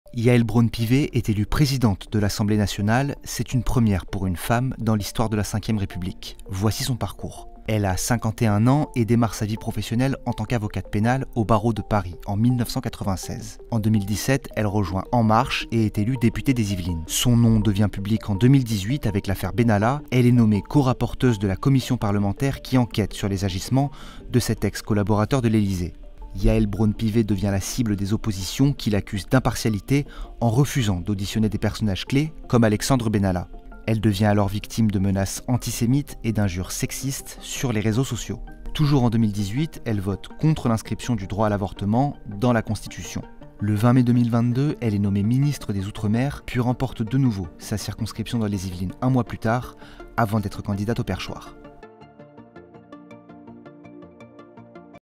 Le parcours de Yaël Braun-Pivet, première femme présidente de l’Assemblée nationale
Les députés ont choisi, mardi 28 juin 2022, la nouvelle présidente de la chambre basse du parlement. Il s'agit de Yaël Braun-Pivet, issue de la majorité présidentielle. Favorite pour ce scrutin, elle va siéger à la place de Richard Ferrand qui n'a pas pu se représenter car battu aux élections législatives dans sa circonscription. Voici son parcours. Elle a 51 ans et démarre sa vie professionnelle en tant qu’avocate en droit pénal au barreau de Paris en 1996. En 2017, elle rejoint En Marche et est élue députée des Yvelines. Son nom devient public en 2018, avec l’affaire Benalla.
Elle est nommée corapporteuse de la commission parlementaire qui enquête sur les agissements de cet ex-collaborateur de l’Élysée  Yaël Braun-Pivet devient la cible des oppositions qui l’accuse d’impartialité, en refusant d’auditionner des personnages clefs, comme Alexandre Benalla. Toujours en 2018, elle vote contre l’inscription du droit à l’avortement dans la Constitution. Le 20 mai 2022, elle est nommée ministre des Outre-mer, puis remporte de nouveau sa circonscription dans les Yvelines, avant d’être candidate au perchoir.